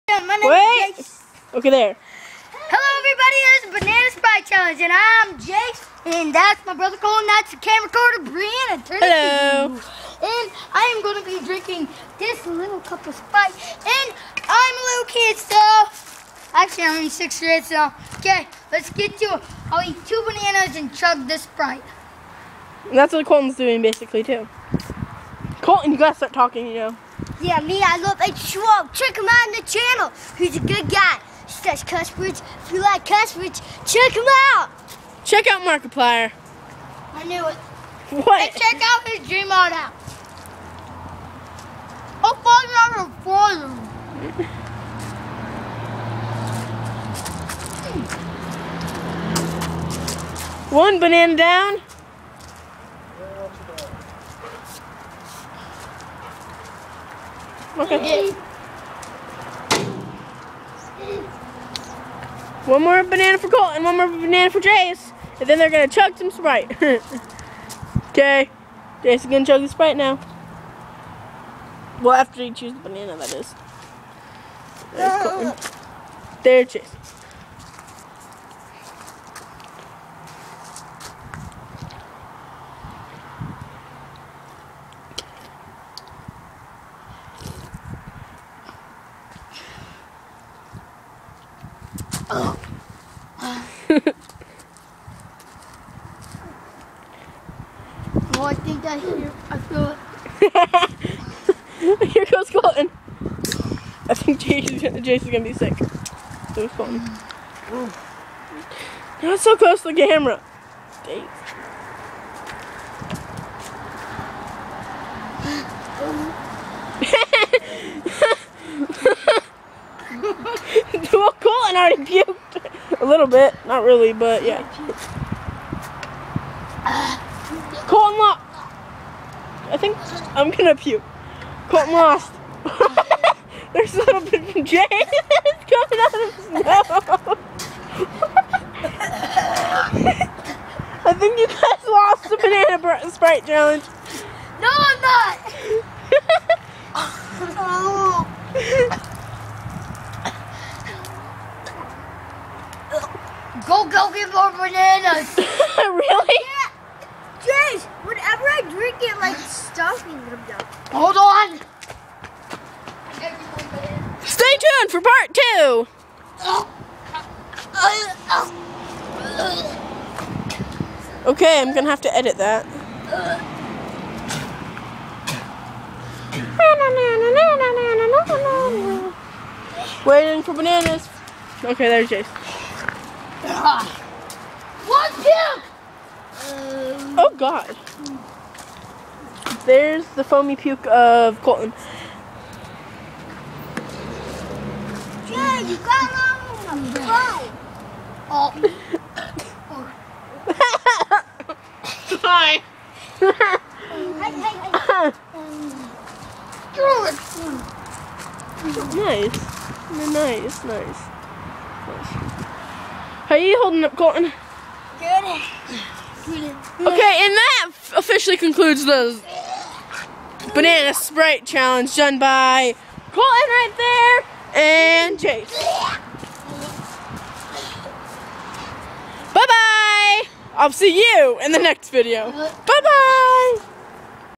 The Banana Sprite Challenge w/ Jace & Colton
My cousins attempt the Banana Sprite Challenge.